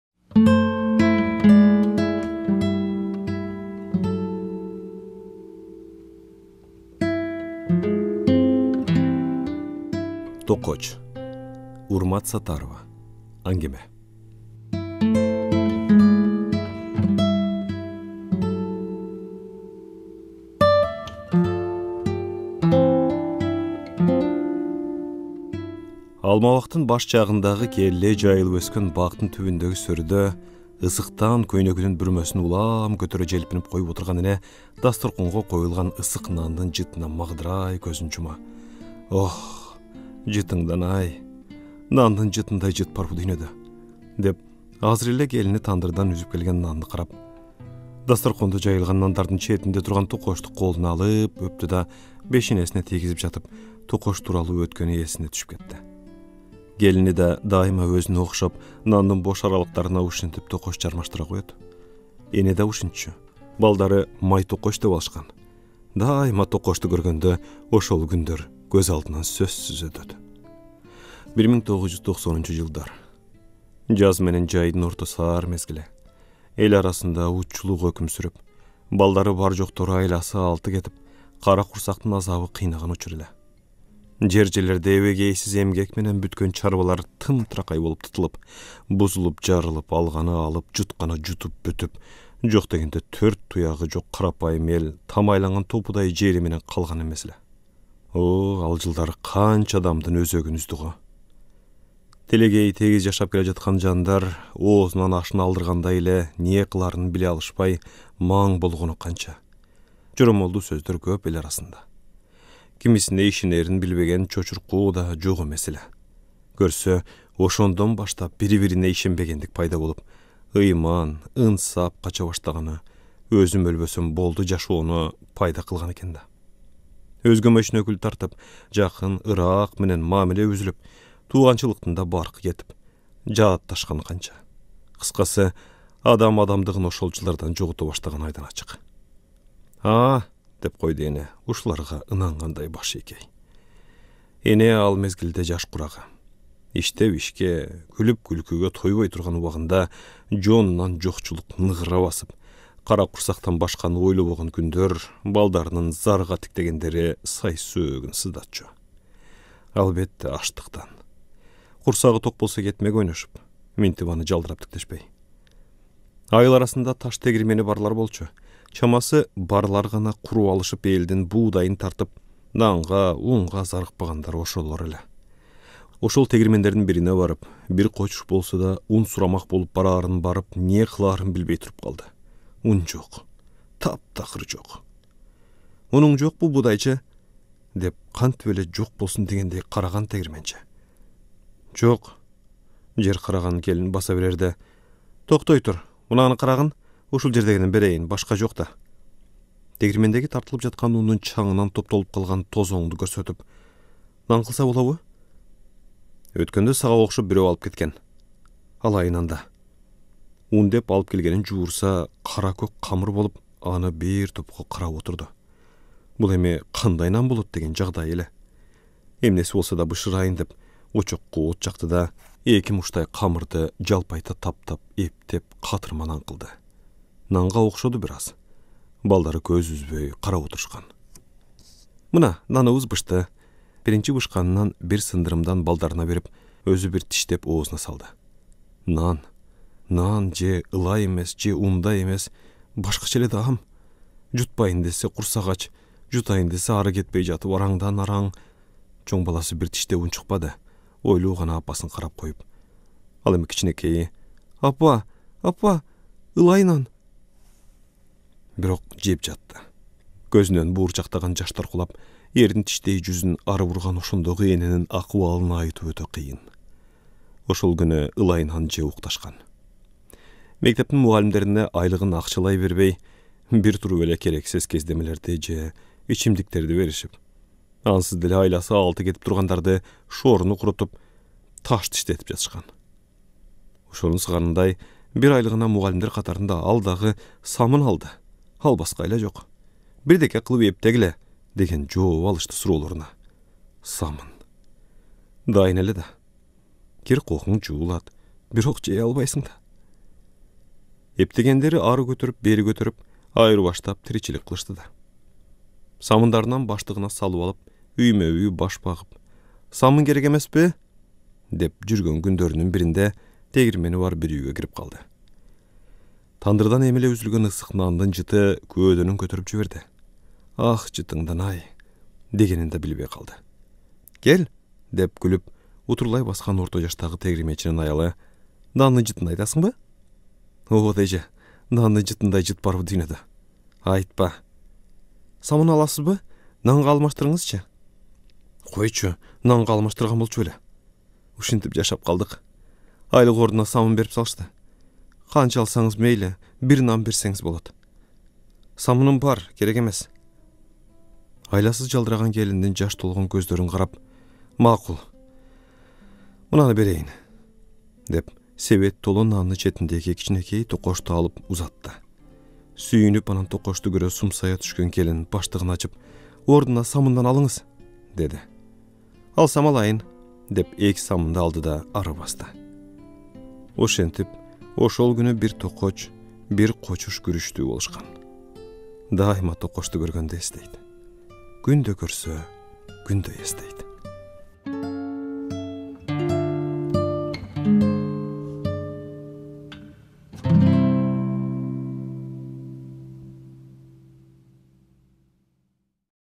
Урмат Сатарова | Токоч | Аңгеме | аудио китеп
1990-жылдардын оор жүгүн ким гана тартпады дейсиң. Элди мурда-кийин көрбөгөн азапка салды го ал жылдар. Менин мектепти аяктап, университетке даярданган, кийин окуй баштаган алгачкы жылдарым. КМУда лекциялардын ортосунда тыныгуу учурунда Филармония тараптагы подземкага чуркап келип, 1 сомго бетине шекер ун чачылган бир туурам нан алып кетчүмүн. Бул - менин түшкү тамагым. Беш сомдук ичинде кичине каймагы бар "лакомкага" акча жетпейт))
Ошентип өткөн ал мезгил. Ал кезде мен жалгыз эмес, бүтүн эл ошондой кыйналды. Ошондой тагдырлардын бирин азыр Москвада турган акын жана жазуучу Урмат Сатарова эже мына бул "Токоч" деген аңгемесинде мыкты сүрөттөгөн экен.